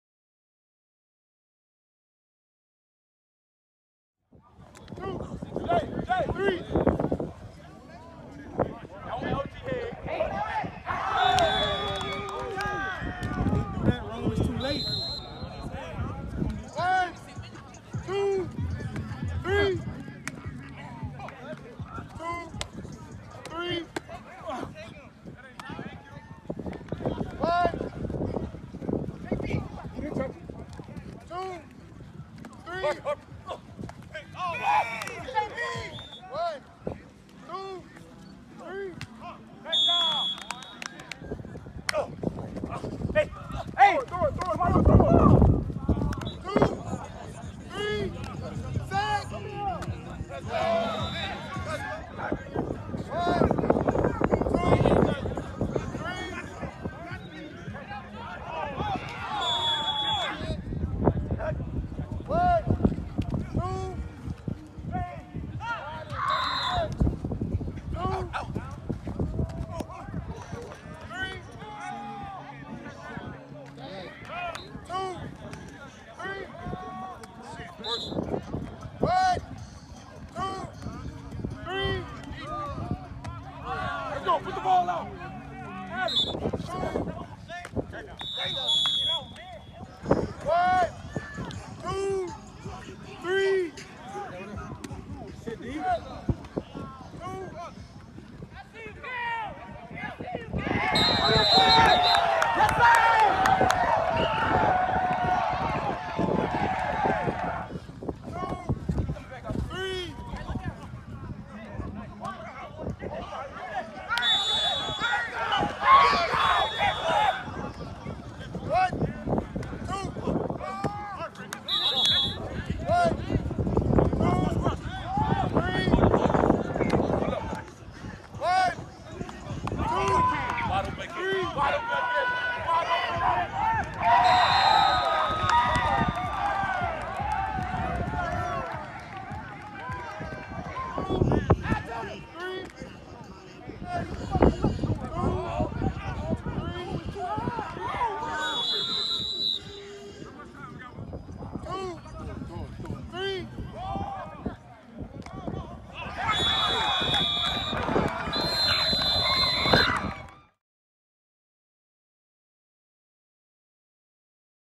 F3 Elite vs. SWLA Bootleggers highlights | Camp Moula Round Robin (Thibodaux, Feb. 27, 2021)
F3 Elite overcomes early interceptions by Laterrance Welch to top Bootleggers' Southwest Louisiana team on strength of Zachary wide receiver Kameran Senegal's dynamic performance.